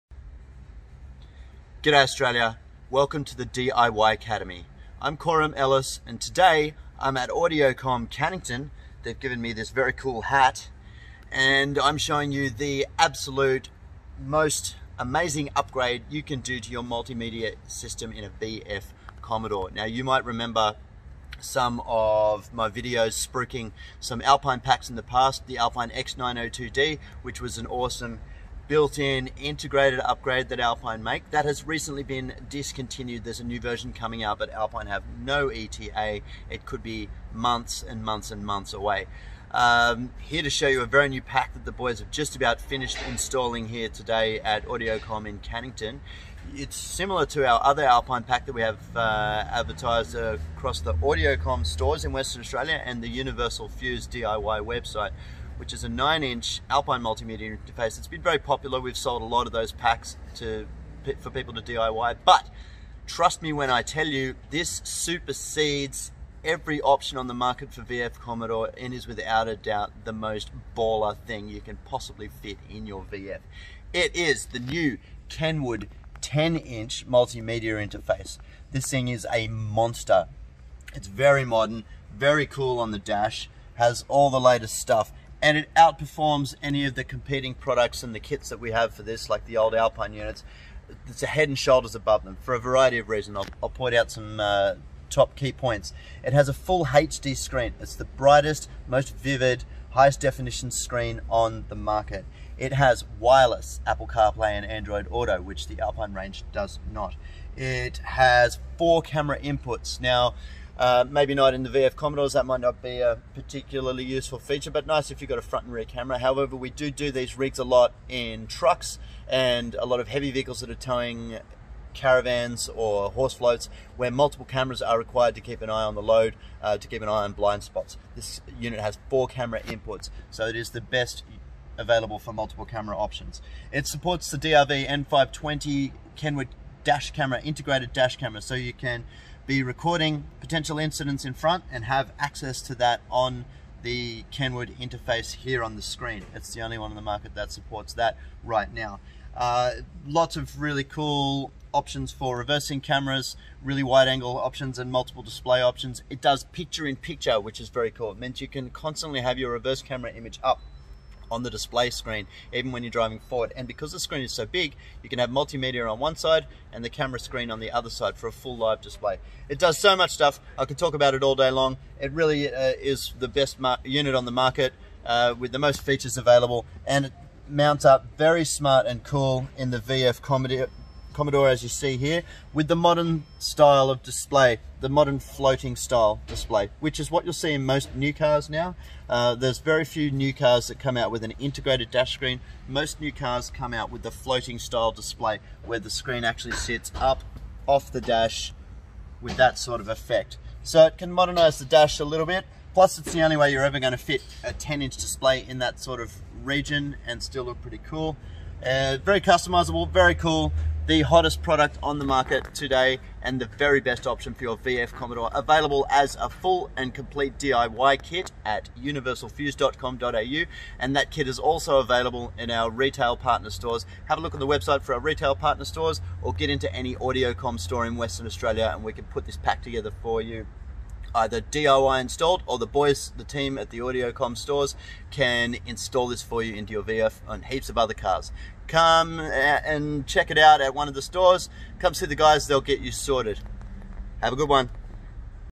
The biggest and best VF Commodore Screen upgrade, Kenwood 10" Multimedia system DMX9720XDS
10 inches is the new everything. For the biggest and best multimedia interface in your Holden VF Commodore that will have your mates frothing, nothing comes close to the performance and majesty of the new Kenwood DMX9720XDS. Available as a DIY pack from Universalfuze or seek pro installation at Audiocom stores in Western Australia.